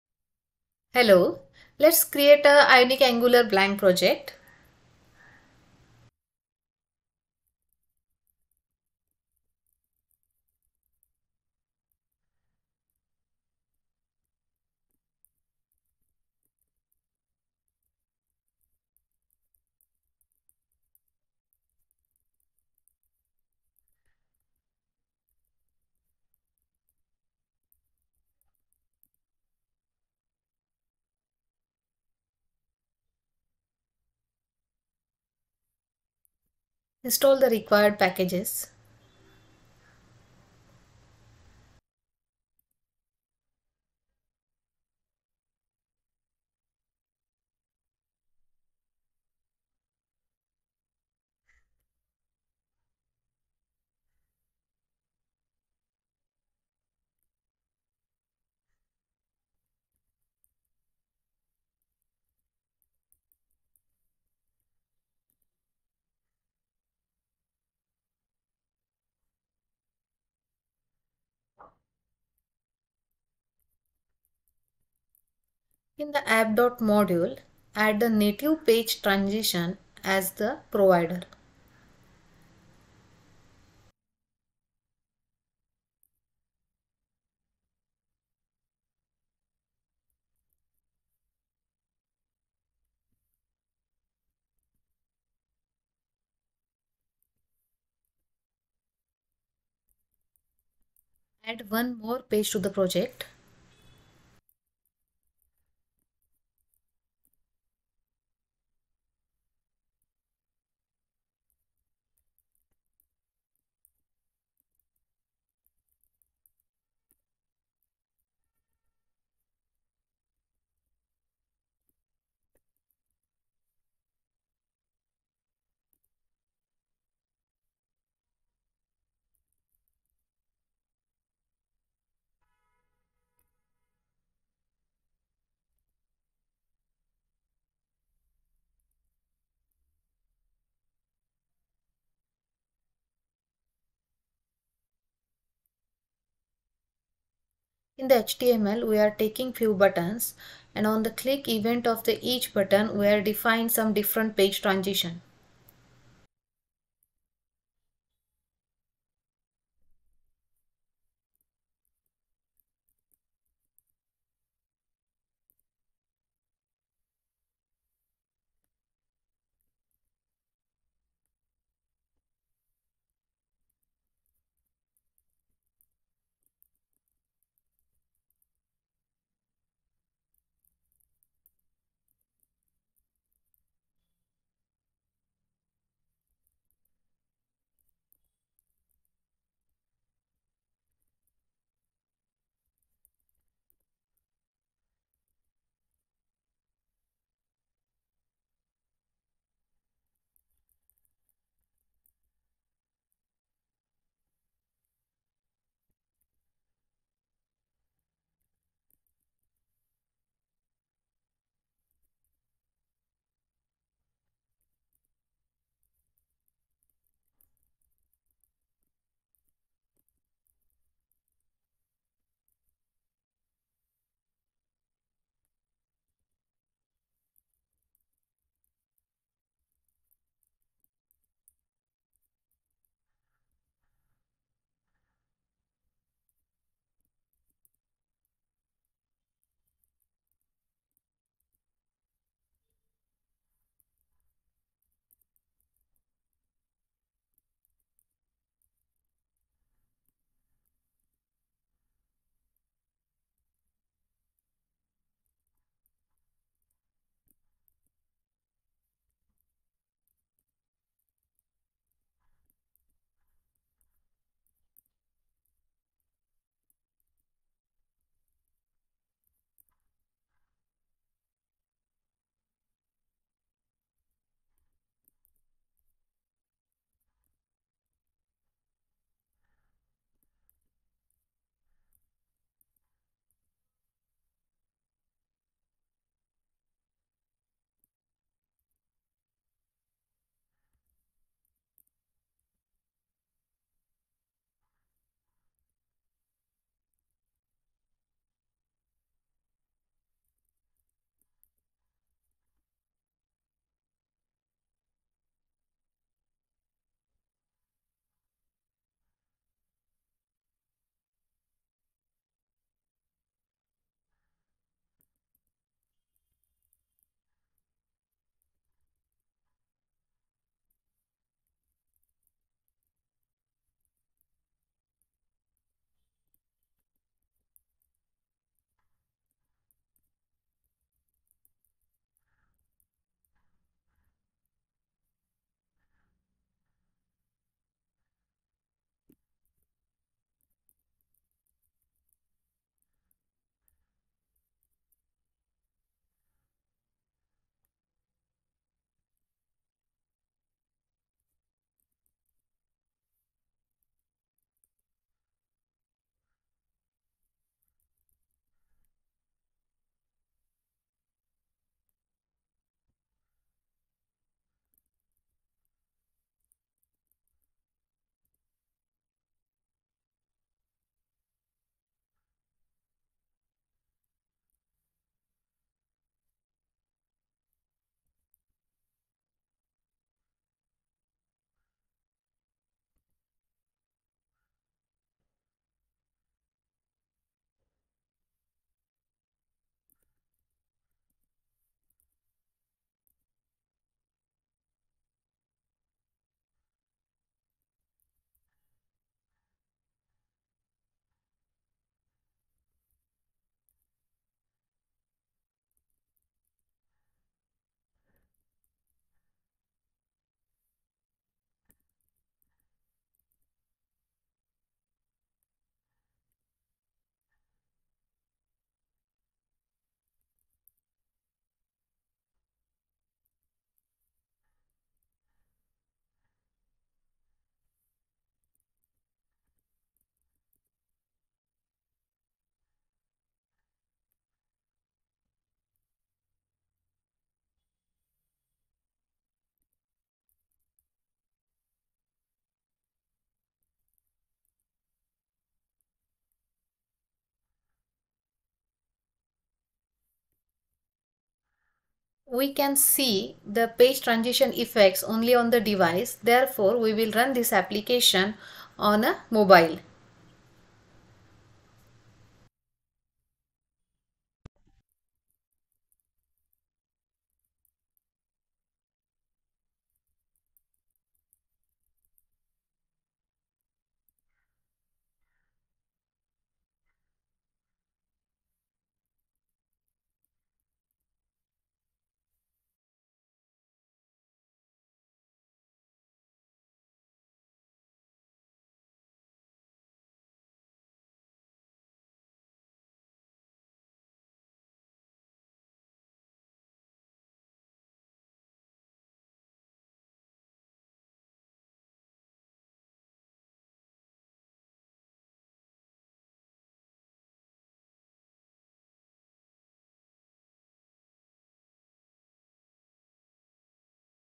Ionic 6 angular Page transitions : slide, flip, drawer, curl, fade
Video will guide you on how to use page transitions in Ionic 6 Angular.

8) Ionic Angular Device API : How to get mobile ID, model, OS version, memory and battery status :